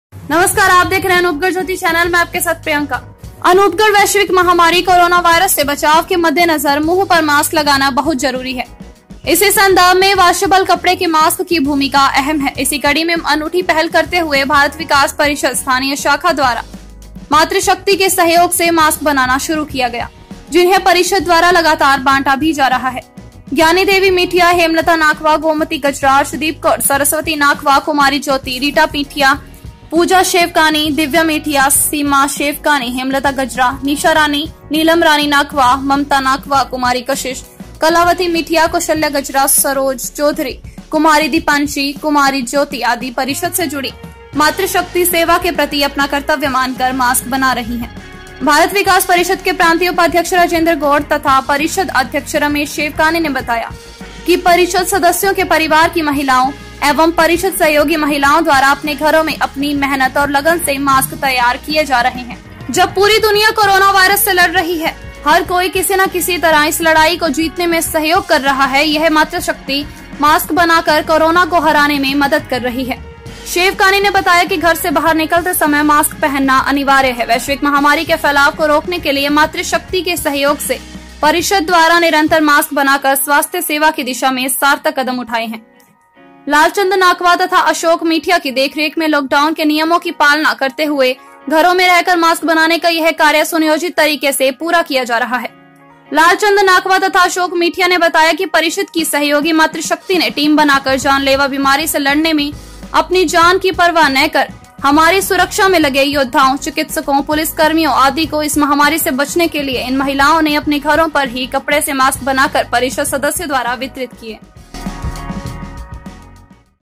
अनूपगढ़: भारत विकास परिषद स्थानीय शाखा द्वारा मातृशक्ति के सहयोग से मास्क बनाना शुरू किया
अनूपगढ़ वैश्विक महामारी करोना वायरस से बचाव के मद्देनजर मुंह पर मास्क लगाना बहुत जरूरी है इसी संदर्भ में वॉशेबल कपड़े के मास्क की भूमिका अहम है इसी कड़ी में  अनूठी पहल करते हुए भारत विकास परिषद स्थानीय शाखा द्वारा मातृशक्ति के सहयोग से मास्क बनाना शुरू किया गया जिन्हें परिषद द्वारा लगातार  बांटा भी जा रहा है।
ज्ञानी देवी मिठिया हेमलता नाखवा गोमती गजरा अर्शदीप कौर सरस्वती नाखवा कुमारी ज्योति रीटा मिठिया  पूजा शेवकानी दिव्या मिठिया सीमा शेवकानी हेमलता गजरा निशा रानी नीलम रानी नाखवा ममता नाखवा कुमारी कशिश कलावती मिठिया कौशल्या गजरा सरोज चौधरी कुमारी दीपांशी  कुमारी ज्योति आदि परिषद से जुड़ी मातृशक्ति सेवा के प्रति अपना कर्तव्य मानकर मास्क बना रही है
भारत विकास परिषद के प्रांतीय उपाध्यक्ष राजेंद्र गौड़ एवं परिषद अध्यक्ष रमेश शेवकानी ने बताया कि परिषद सदस्यों के परिवार की महिलाओं एवं परिषद सहयोगी महिलाओं द्वारा अपने घरों में अपनी मेहनत और लगन से मास्क  तैयार किए जा रहे हैं। जब पूरी दुनिया करोना से लड़ रही है हर कोई किसी ना किसी तरह इस लड़ाई को जीतने में सहयोग कर रहा है यह मातृशक्ति मास्क बनाकर करोना को हराने में मदद कर रही है  शेवकानी ने बताया कि घर से बाहर निकलते समय  मास्क पहनना अनिवार्य है वैश्विक महामारी के फैलाव को रोकने  के लिए मातृशक्ति  के सहयोग से  परिषद द्वारा  निरंतर  मास्क बनाकर स्वास्थ्य सेवा की दिशा में सार्थक कदम उठाए हैं लालचंद नाखवा एवं अशोक मिठिया की देखरेख में लॉक डाउन के नियमों की पालना करते हुए घरों में रहकर मास्क बनाने का यह कार्य सुनियोजित तरीके से पूरा किया जा रहा है।
लालचंद नाखवा  एवं अशोक मिठिया  ने बताया कि परिषद की  सहयोगी मातृशक्ति ने टीम बनाकर जानलेवा बीमारी से लड़ने मैं अपनी जान की परवाह न कर हमारी सुरक्षा में लगे योद्धाओं चिकित्सकों पुलिसकर्मियों आदि को इस महामारी से बचने के लिए इन महिलाओं ने अपने घरों पर ही कपड़े से मास्क बनाकर परिषद सदस्यों द्वारा वितरित किए जाएंगे